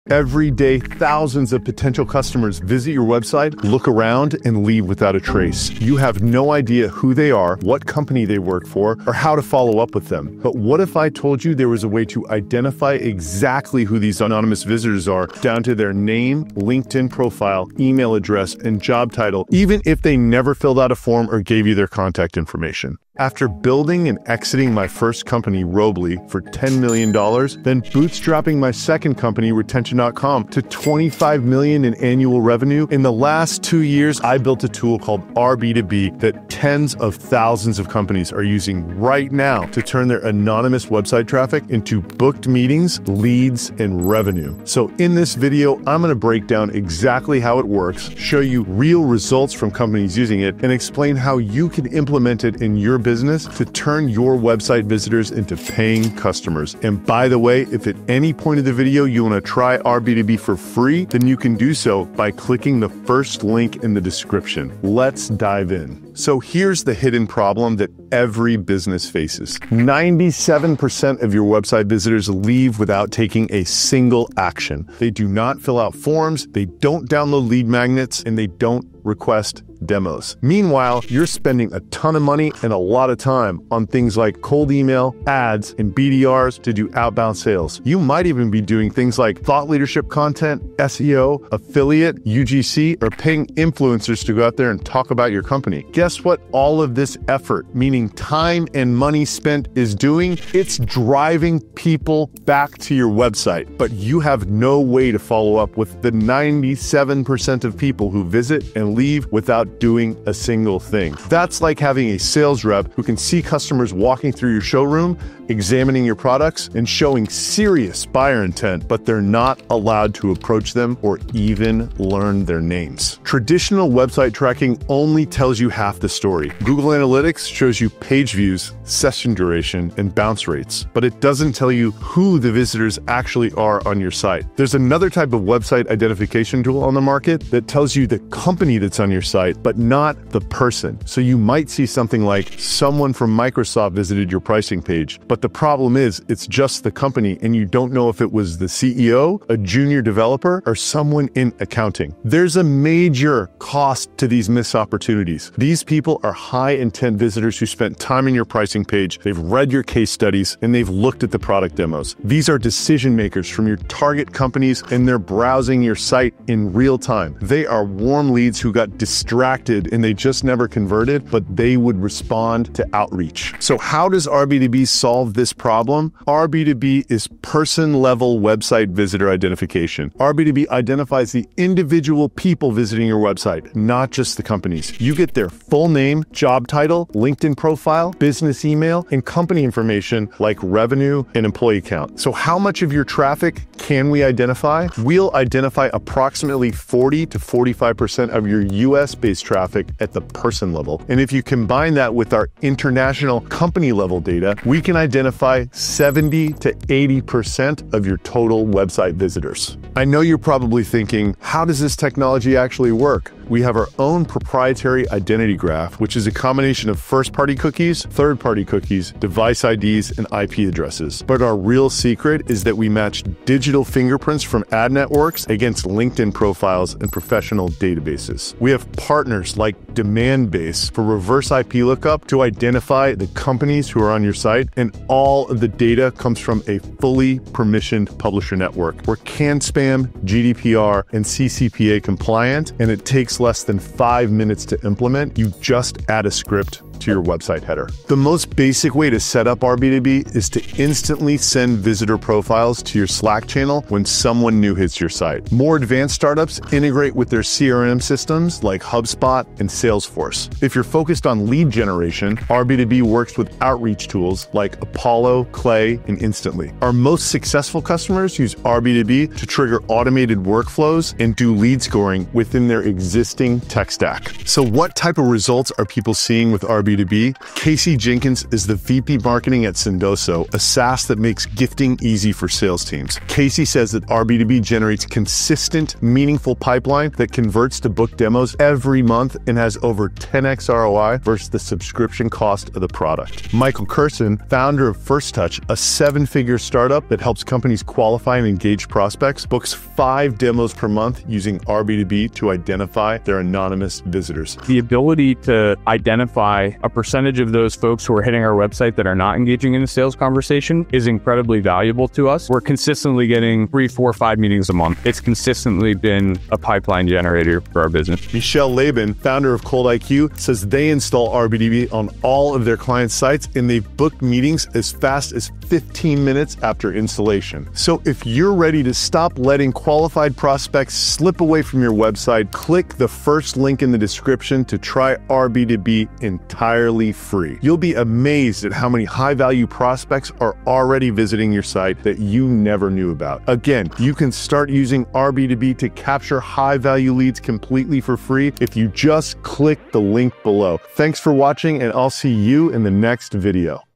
How to Identify & Contact Your Website Visitors (2025)
In this video, I show you exactly how you can identify and contact your website visitors using RB2B.